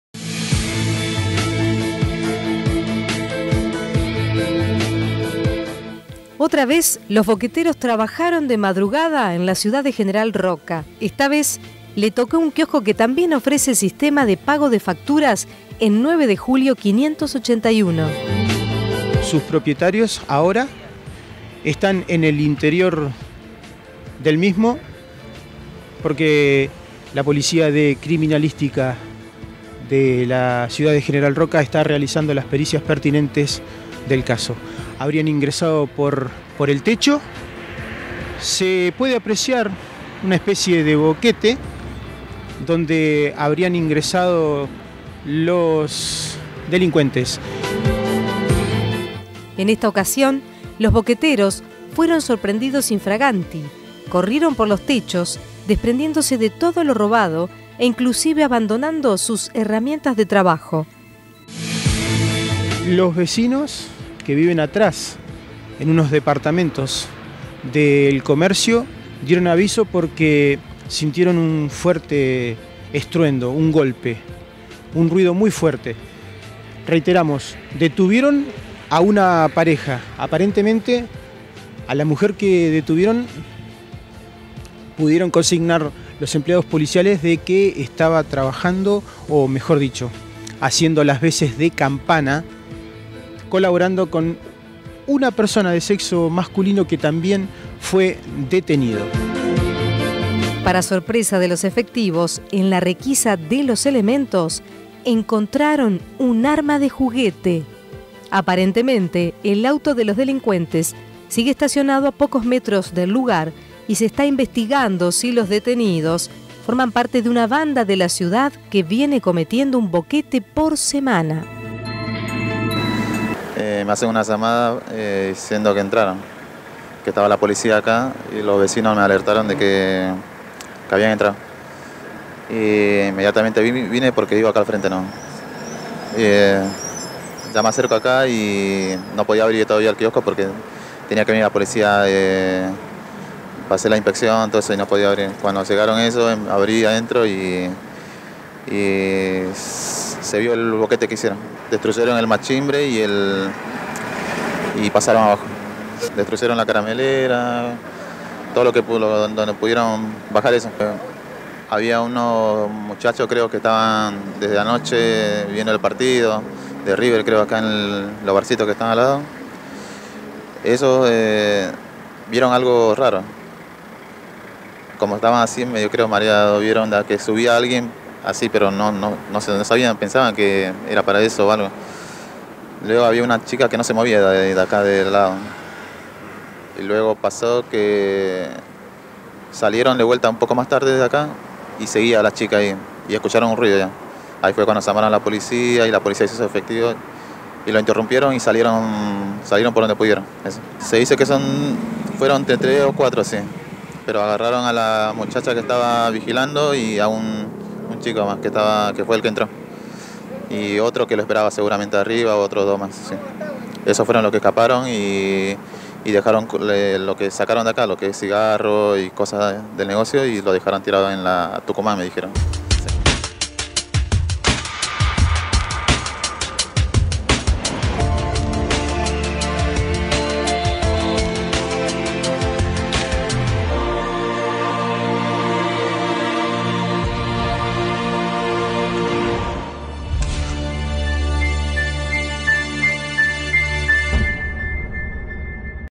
Roca: Atrapan a boqueteros in fraganti
Dos jóvenes fueron detenidos esta mañana por personal de la Comisaría Tercera al hallarlos intentando robar a un kiosco con "Pago Fácil" ubicado en 9 de Julio e Italia. Habían comenzado a hacer un boquete en el techo del local comercial con la ayuda de herramientas en plena oscuridad. Los jóvenes tienen 26 y 15 años.